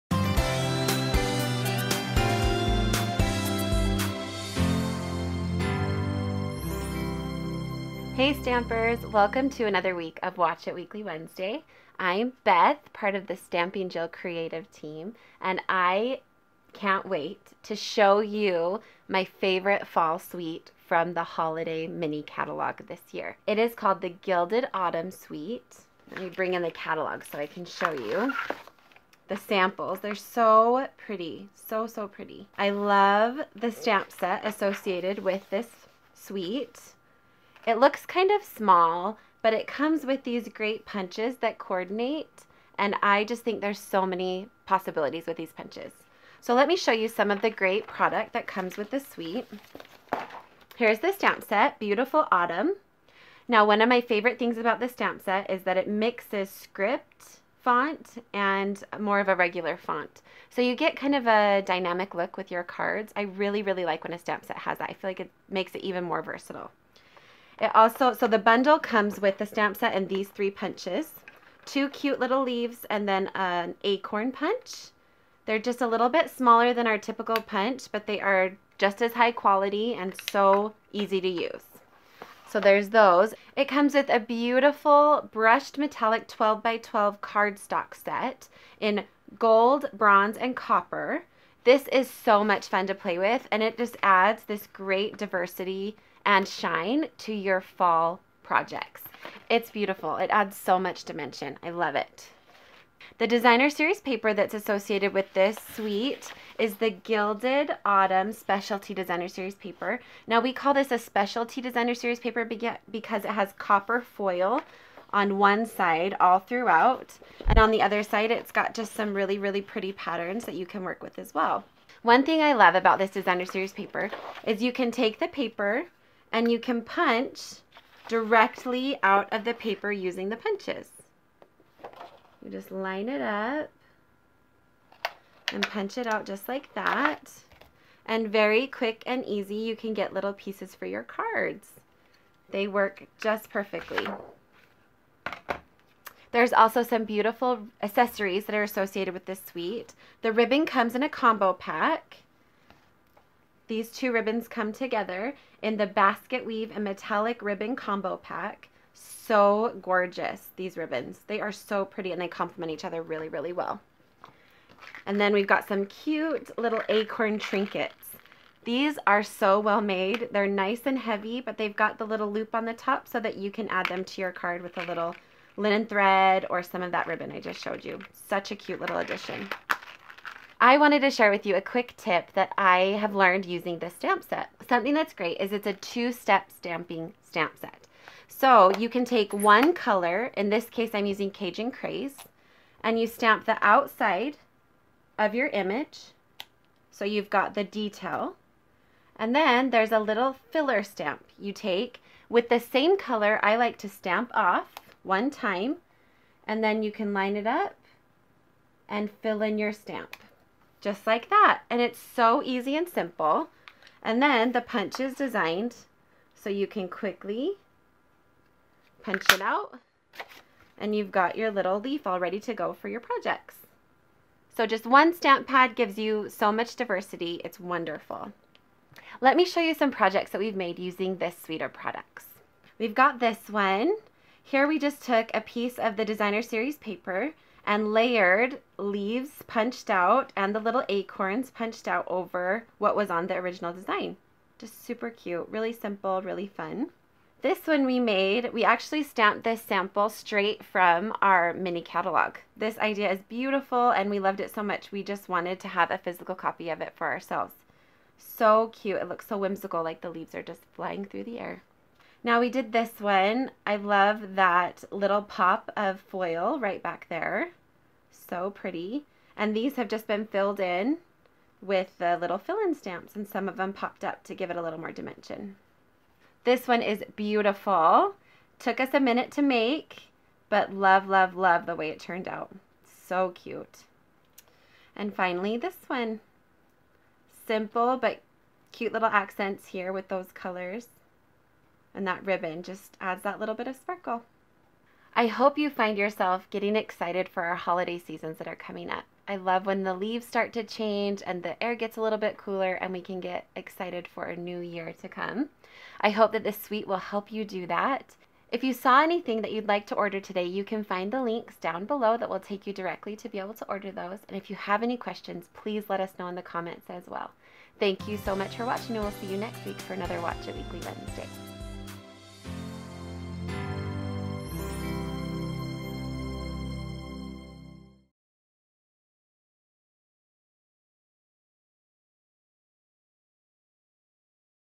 Stamping Jill - Gilded Autumn Suite Reveal & Card Ideas
Stampin' Up! Demonstrator Beth demonstrates the Gilded Autumn Suite and beautiful card ideas. This suite
is perfect for anytime of year but especially Fall. With the designer series paper and punches that are included in this suite it makes for an awesome bundle. Make sure you check it out here!